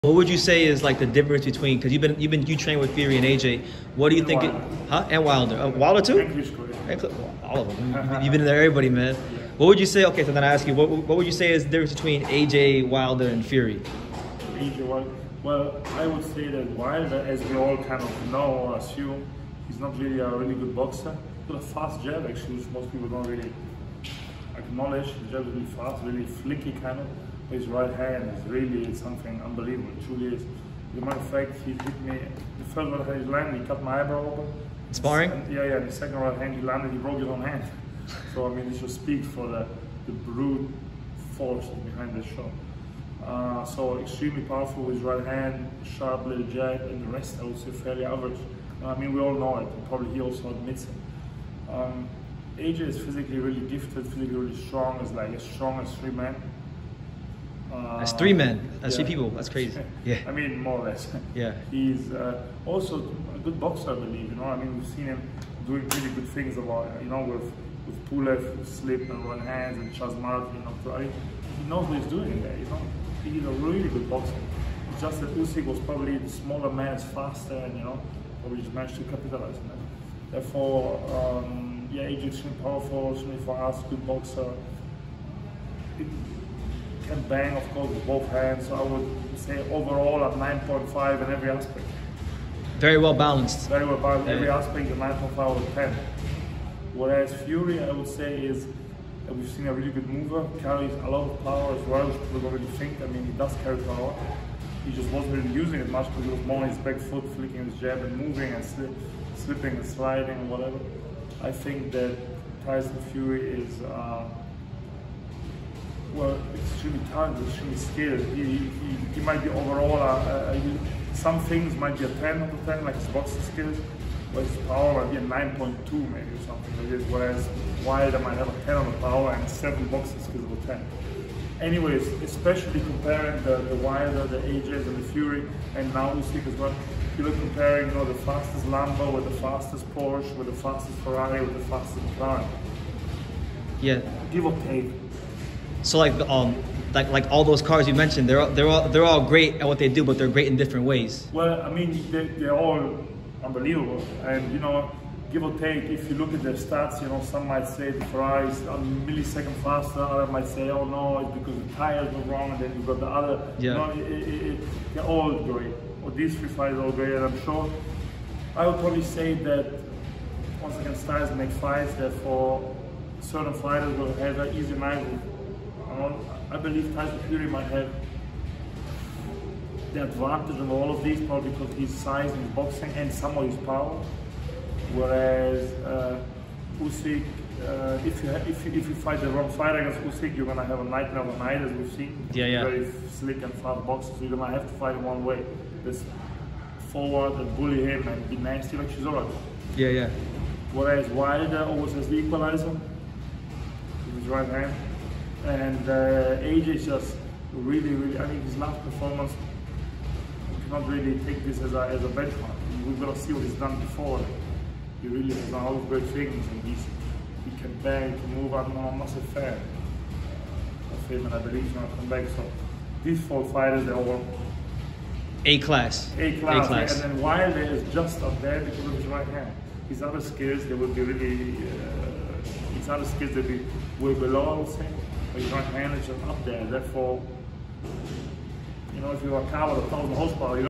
Christian Thun Explains Differences Sparring With Tyson Fury Deontay Wilder & Anthony Joshua
Christian Thun is an experienced Heavyweight prospect and he’s had the honor of sharing the ring with Tyson Fury Deontay Wilder & Anthony Joshua. In this video he tells us the differences in each fighters style that makes them successful in boxing. It’s a lovely breakdown and Christian is gonna be a name you hear about very soon in heavyweight boxing.